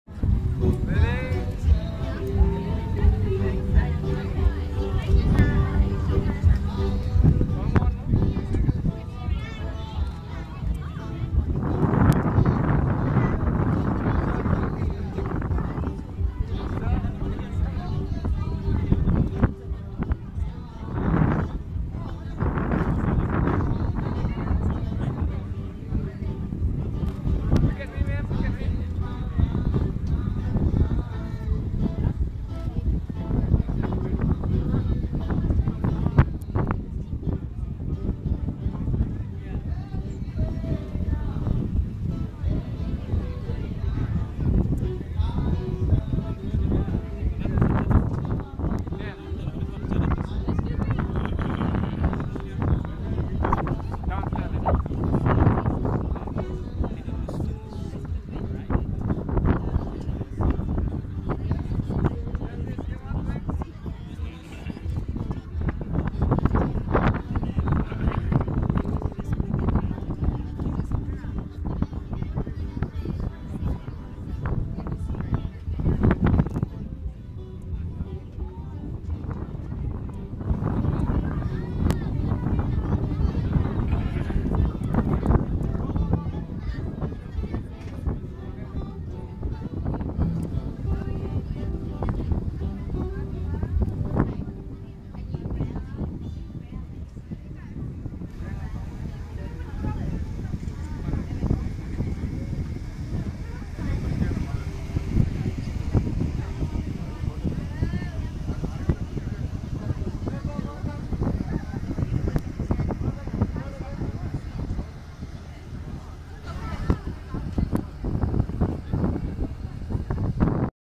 Bali -Golden Sunset🌝🌖captured from the deck of Cruise🌞aroundtheglobe
This Video is made during a Journey in Cruise at Bali. The Evening was really Like a Shining Gold